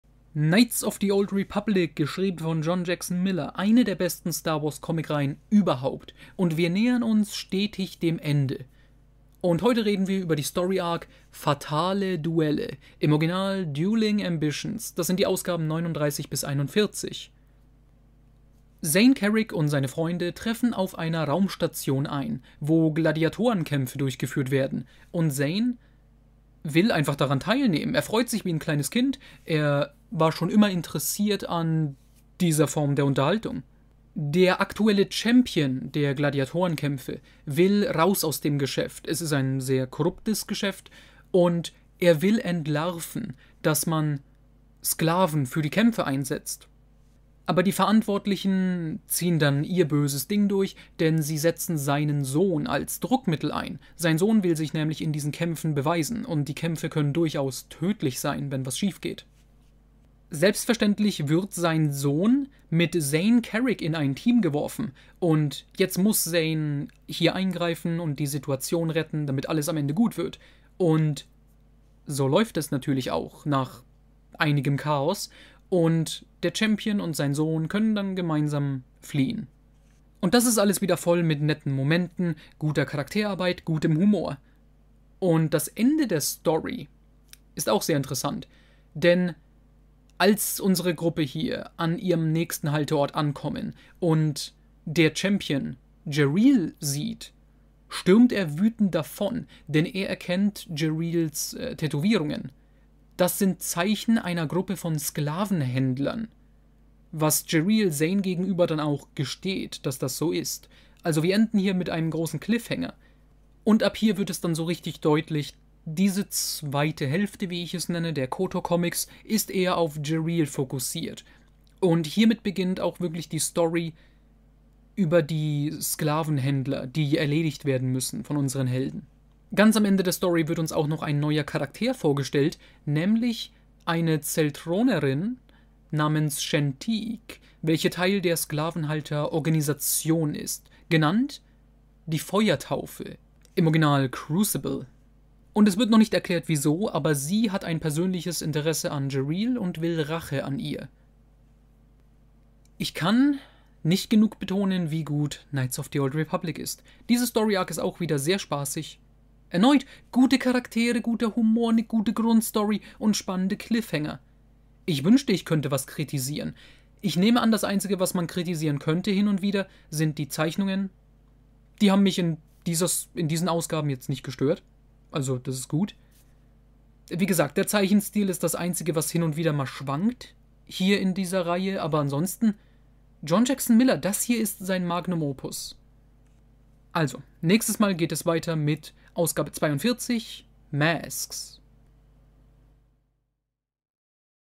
Star Wars Legends: Knights of the Old Republic: Fatale Duelle
"Fatale Duelle" (Dueling Ambitions), die fünfzehnte Story in der Reihe Knights of the Old Republic von John Jackson Miller.